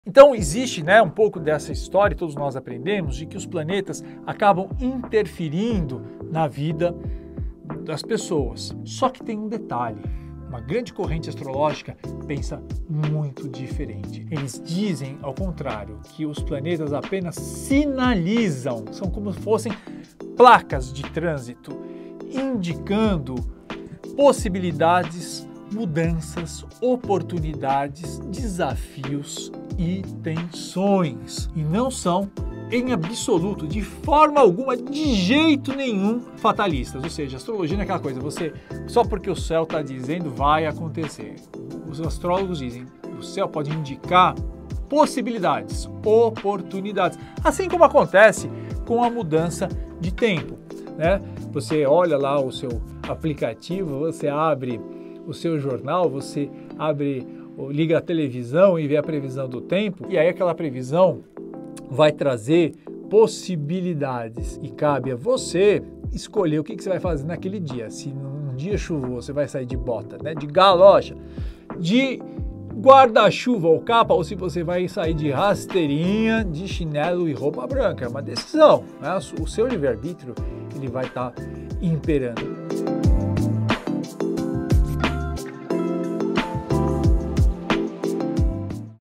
Astrologia e livre arbítrio
Confrontando mitos astrológicos e aprendendo a ler sinais do céu, em que o livre-arbítrio é rei. Neste curso, vamos entender o que é astrologia, o que é livre-arbítrio e como ele é soberano. O céu pode trazer informações sobre oportunidades e tensões, mas somos nós os senhores de nossos destinos. Aprender a ler a Natureza em nossa trajetória é como ler placas de trânsito em uma estrada. Há indicativos para que a viagem possa ser mais segura e tranquila.

😍 Conheça: @ricardohida.oficial